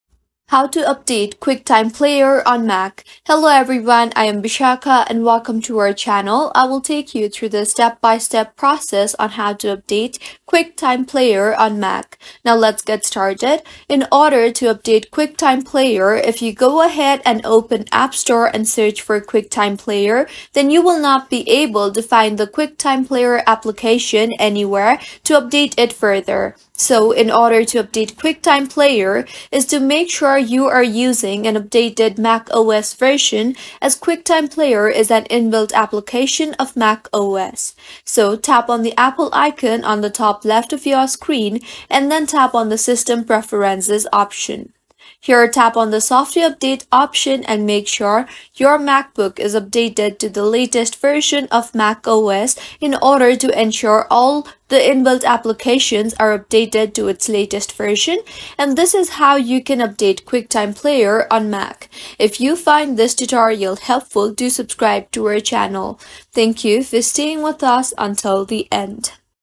How To Update QuickTime Player on Mac (EASY)
Is your QuickTime Player on Mac in need of an update? In this tutorial, we'll show you the simple steps to ensure you have the latest version for the best performance.

Learn why keeping your QuickTime Player up to date is essential and how it can enhance your Mac experience.

Step-by-Step Tutorial:

[00:00] Introduction and Importance of Updates
[00:10] How To Update QuickTime Player on Mac

Follow our concise instructions to keep your QuickTime Player current and make the most of its features.